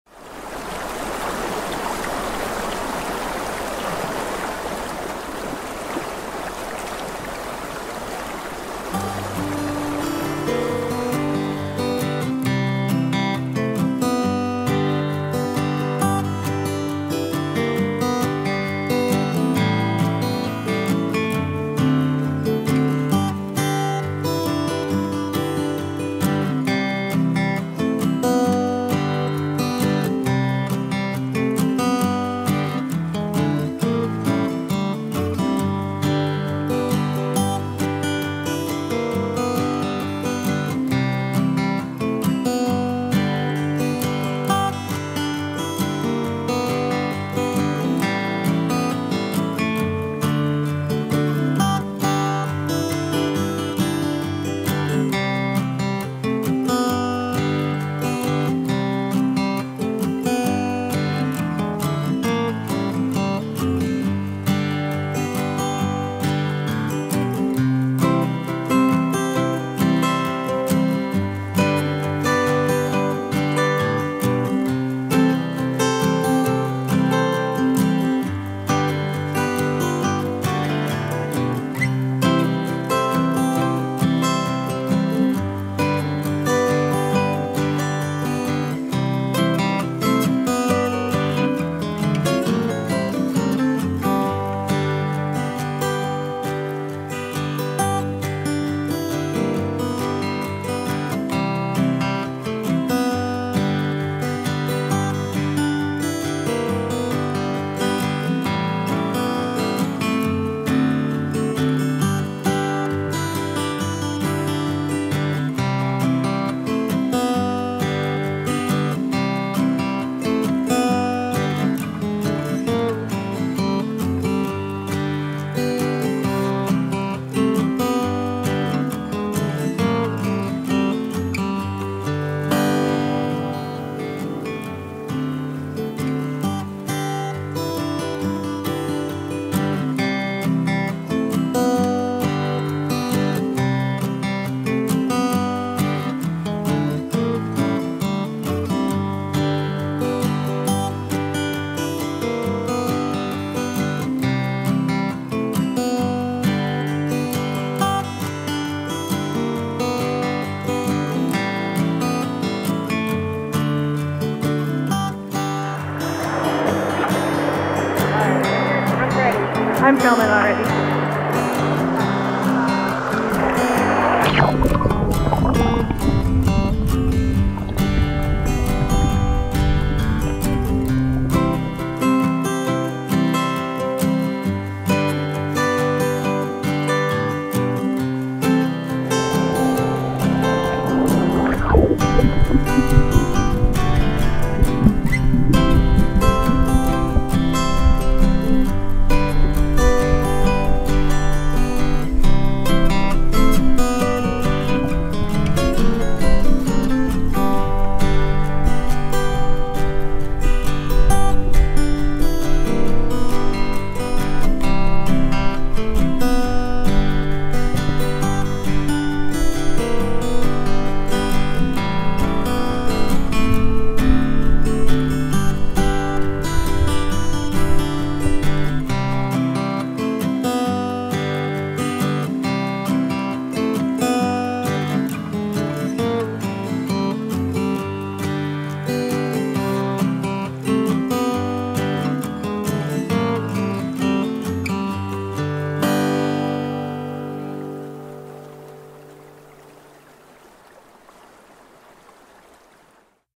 Release the Fish!
The fifth and final installment in our Trout Week! Each day this week, the Naturalist Educators posted a new video about trout, so make sure to catch them all.

This is a fun video of our trout release at Oxbow Nature Study Area!